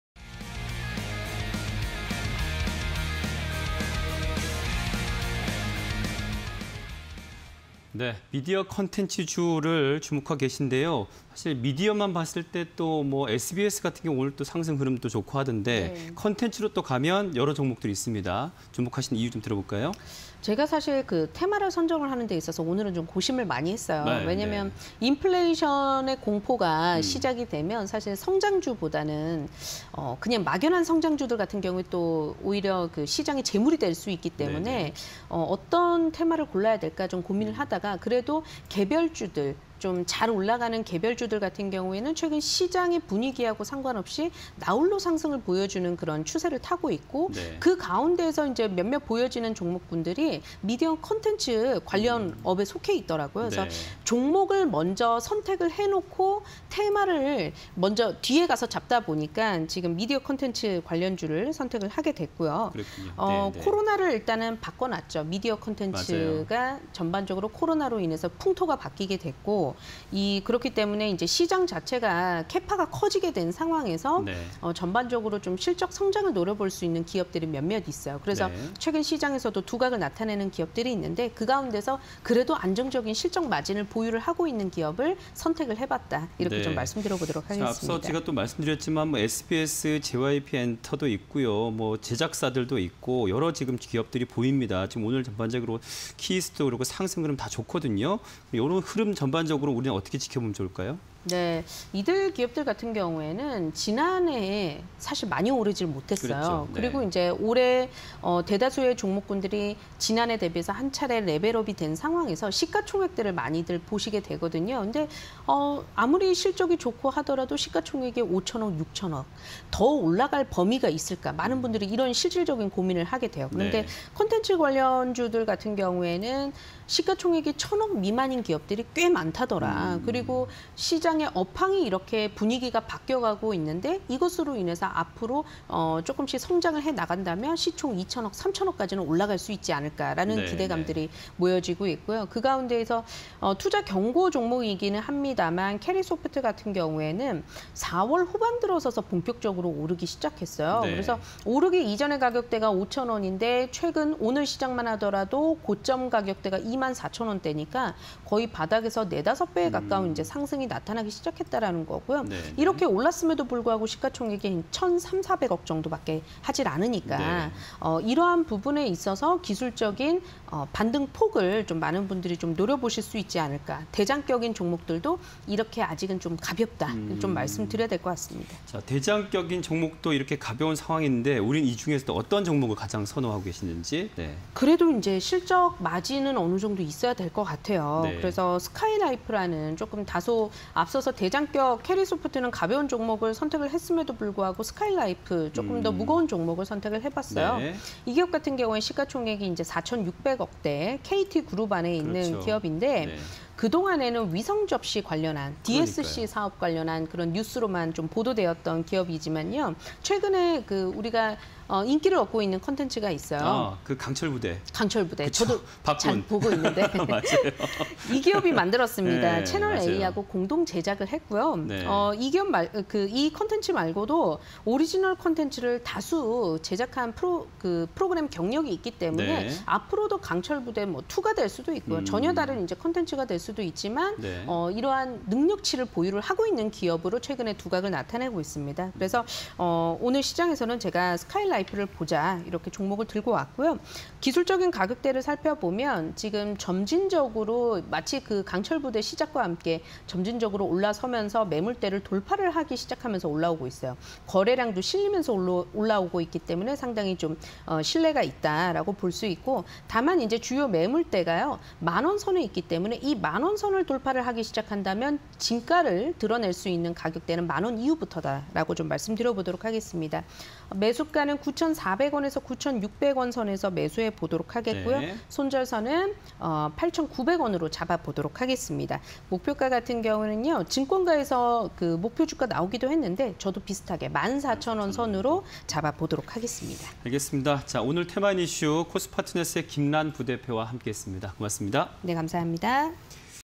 스카이라이프(053210)... 미디어 콘텐츠주_테마IN이슈 (20210517)
미디어 콘텐츠주
- 코로나19가 바꿔놓은 미디어 콘텐츠 '풍토'
- 미디어 콘텐츠 시장 확대… 성장 프리미엄

스카이라이프(053210)
- KT그룹, 3월 미디어 콘텐츠 사업 전략 발표
- KT그룹 내 미디어 콘텐츠 '구심점' 역할
- 8개 채널 운영ㆍ오리지널 콘텐츠 제작 경험
- 채널A와 공동제작 '강철부대' 인기몰이